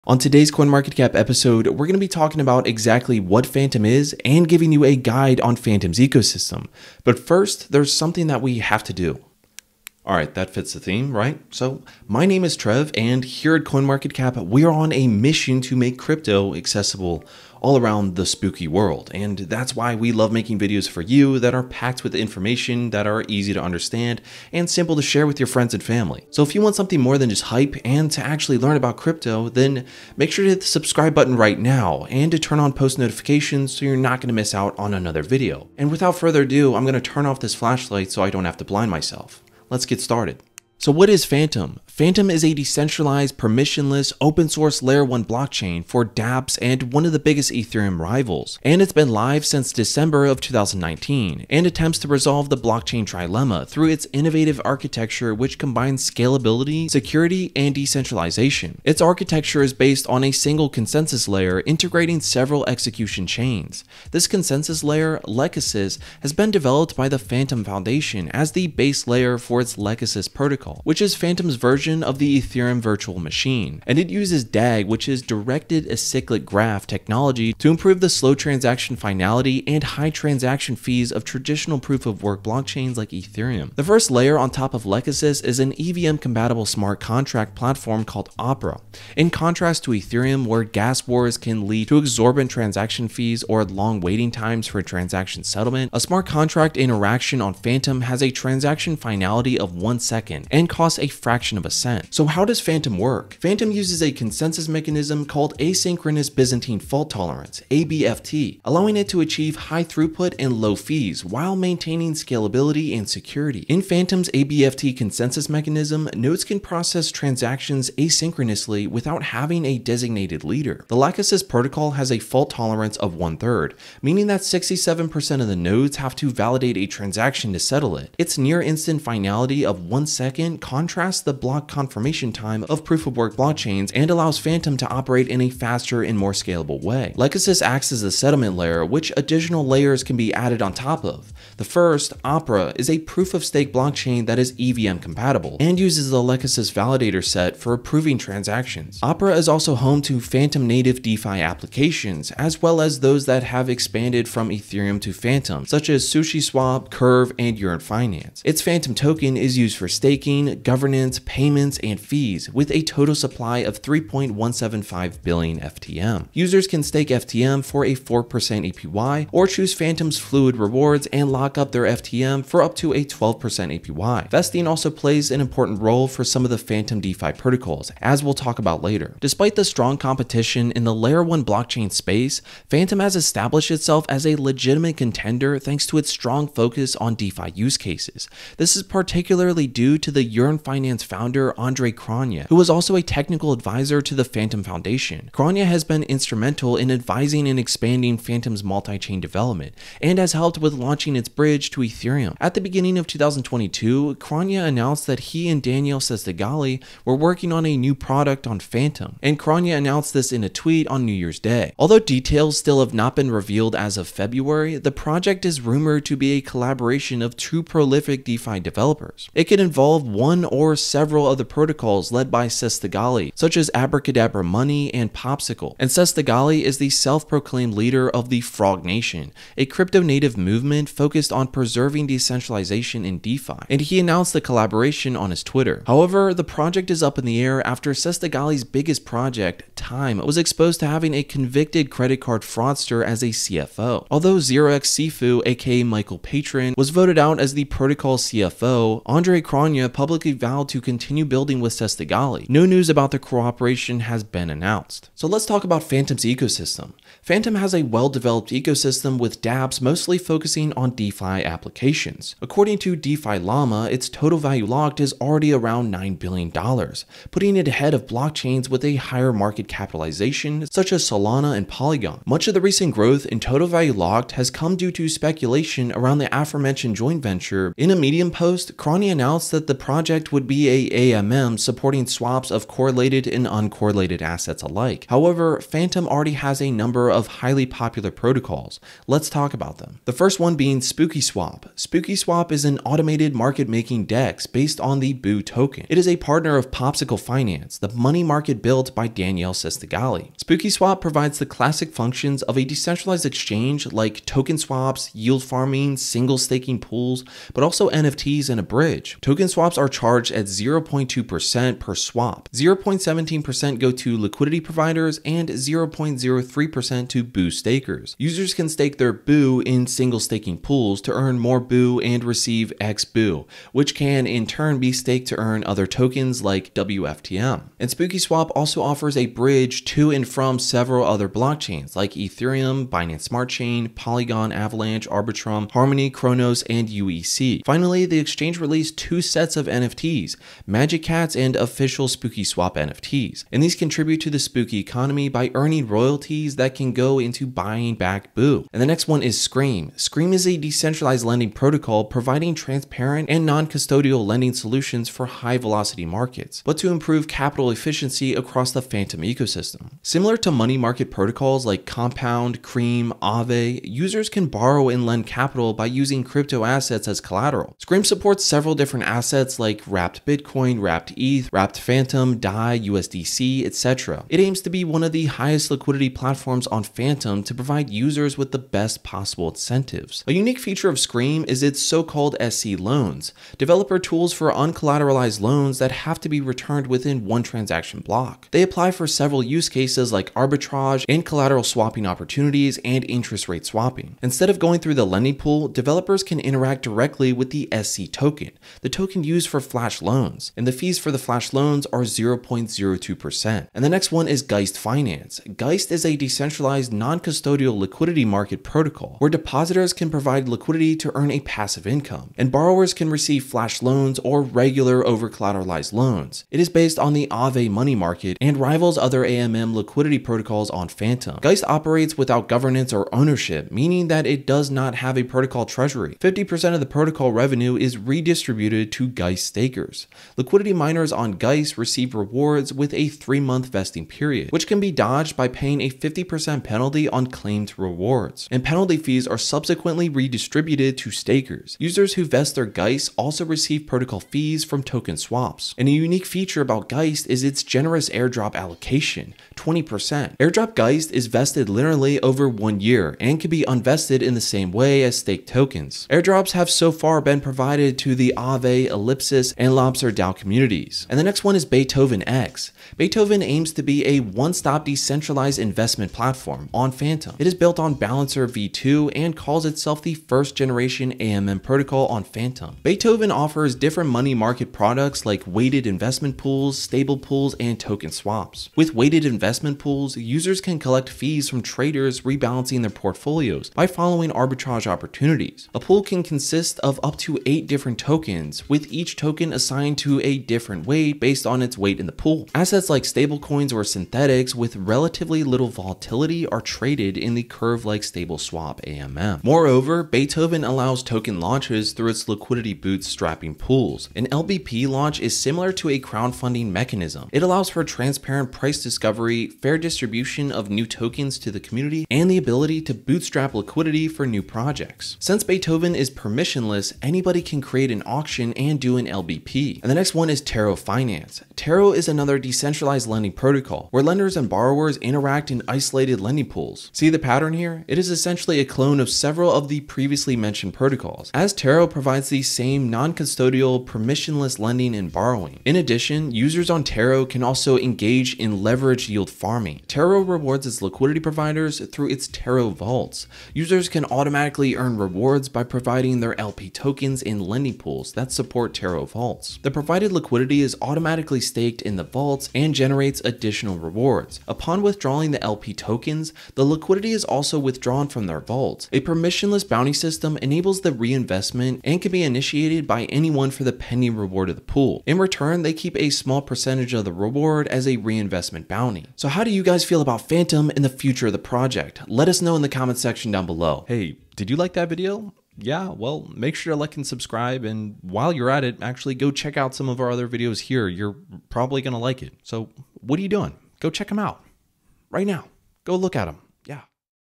What Is Fantom? [ FTM Explained ]
CoinMarketCap takes a look at a promising layer-1 blockchain. Grab a blanket if you’re scared of ghosts and spooky stories because we’re focusing on Fantom today!

Fantom is a decentralized, permissionless, open-source layer-1 blockchain for dApps and one of the biggest Ethereum’s rival. It has been live since December 2019 and attempts to resolve the blockchain trilemma through its innovative architecture, which combines scalability, security, and decentralization.

Its architecture is based on a single consensus layer integrating several execution chains. This consensus layer – Lachesis – has been developed by the Fantom Foundation as the base layer for its Lachesis Protocol, Fantom’s version of the Ethereum Virtual Machine. It uses DAG (Directed Acylic Graph) technology to improve the slow transaction finality and high transaction fees of traditional Proof-of-Work blockchains like Ethereum.

The first layer on top of Lachesis is an EVM-compatible smart contract platform called Opera. In contrast to Ethereum, where gas wars can lead to exorbitant transaction fees or long waiting times for transaction settlement, a smart contract interaction on Fantom has a transaction finality of one second and costs a fraction of a cent.